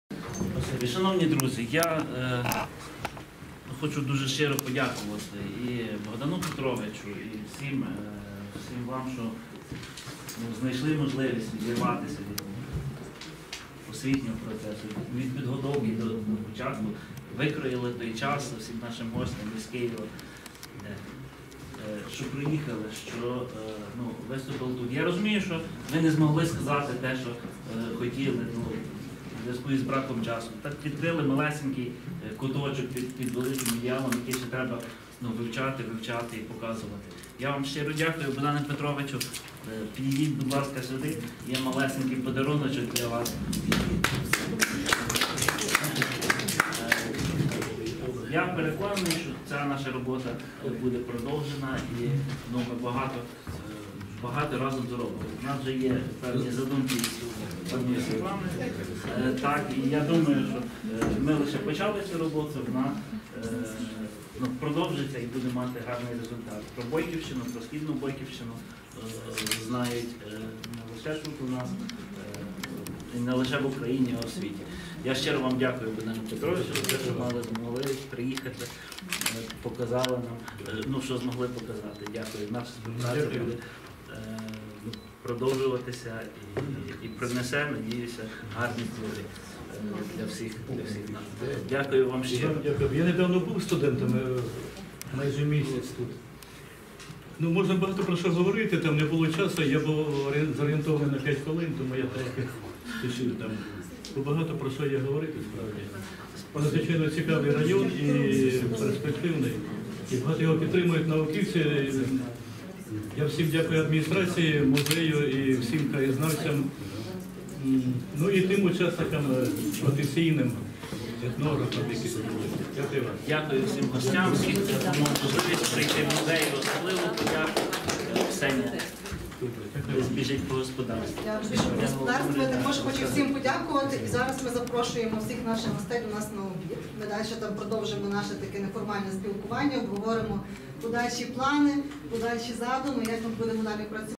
До підсумків Конференції був запрошений міський голова Іван Дирів.
Зокрема він наголосив на тому, що бойки мають багато всього, що потрібно ще відкрити, про що потрібно говорити, щоб про це дізналися якомога більше людей, а особливо тих, в яких бойківське коріння, але зовсім мало інформації про рід й родовід, малу батьківщину. Не оминув і туристичної складової, адже на нинішньому сесійному засіданні було внесено (і підтримано депутатами) на участь проект “Замчище. Відновлення історичної ідентичності” у дванадцятому обласному конкурсі проєктів та програм розвитку місцевого самоврядування.

Довідково:
27 серпня у краєзнавчому музеї “Бойківщина» Тетяни і Омеляна Антоновичів відбулась Регіональна науково-практична конференція «Терени Східної Бойківщини. Хто такі бойки?»
Наукового спрямування захід Долинська міська рада організувала в рамках реалізації проєкту “Створення системи комплексних заходів для розвитку туризму у Долинській ОТГ” одинадцятого обласного конкурсу проєктів та програм розвитку місцевого самоврядування.
Окрім того, це свого роду змістовна передмова до Бойківського міжнародного інвестиційного Форуму «Експо 2021». Продовженням  було проведення 28 серпня  першого етнографічно – гастрономічного Фестивалю «Ситий Бойко» в селі Грабові.